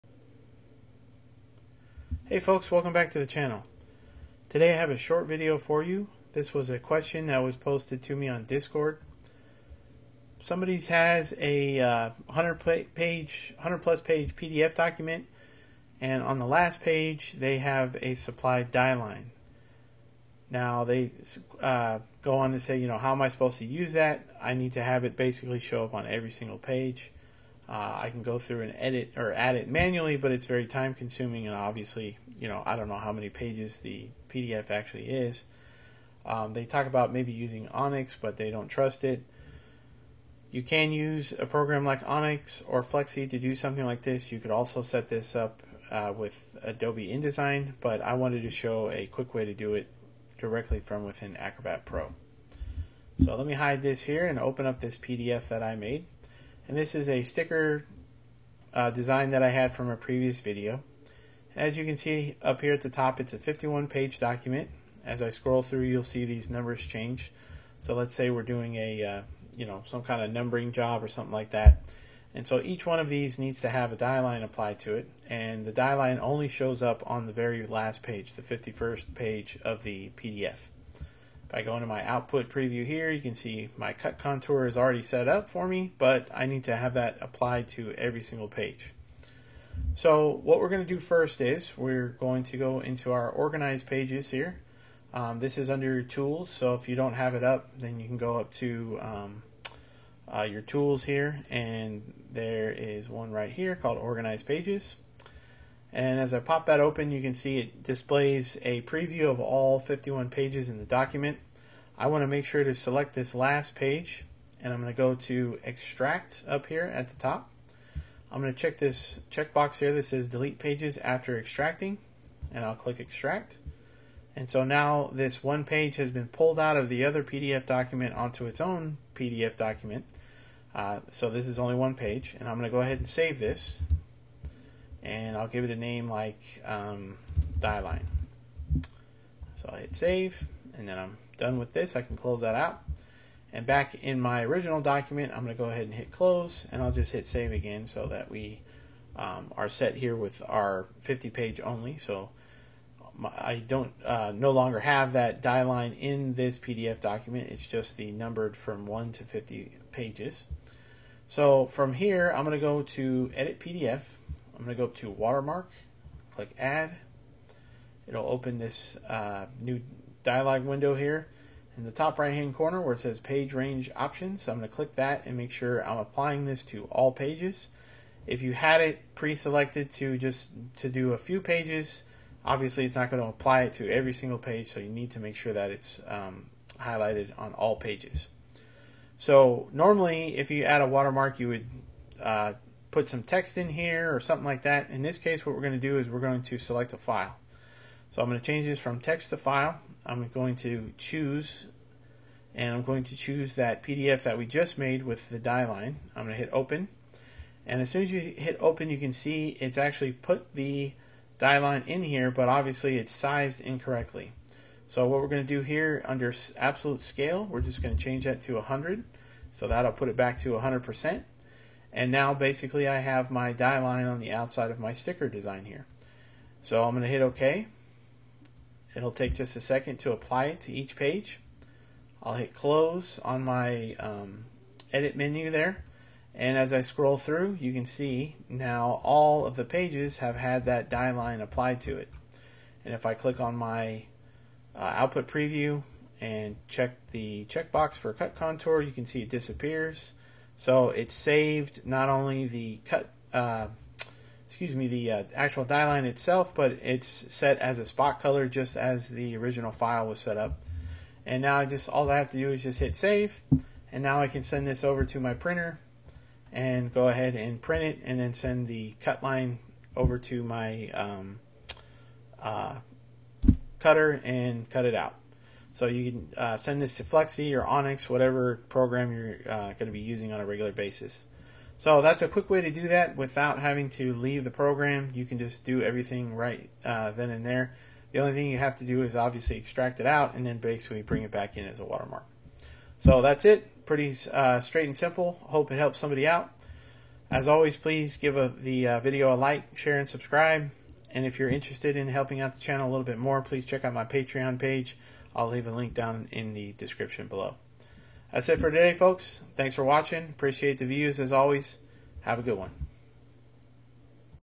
How to Insert an Existing Die Line into a PDF
Got a 100-page PDF but the die-line is only on the last page? Don't waste hours copy-pasting or manually placing files in InDesign. In this tutorial, I show you a prepress "hack" to automatically apply a die-line to every single page of a PDF document directly inside Adobe Acrobat Pro.

This method is perfect for sticker runs, variable data numbering jobs, or any print project where you need a consistent cut path across multiple pages.

In this video, you’ll learn:
01:30 Extracting the Die-Line: How to isolate the cut path onto its own single-page PDF.
02:40 The Watermark Hack: Why the "Add Watermark" tool is the fastest way to batch-apply artwork to every page.
03:00 Settings for Success: Ensuring the watermark applies to the full page range at 100% scale.
04:05 Verifying the Spot Color: Using Output Preview to ensure your "CutContour" or "DieLine" spot color remains intact for your RIP software (Onyx, Flexi, etc.).
04:30 Production Ready: Saving your file and preparing it for your printer and digital cutter.

Why use Acrobat for this? While you could use InDesign or a RIP like Onyx, this method is significantly faster for simple overlays. It keeps your spot colors intact and ensures perfect registration across the entire document without leaving the Acrobat environment.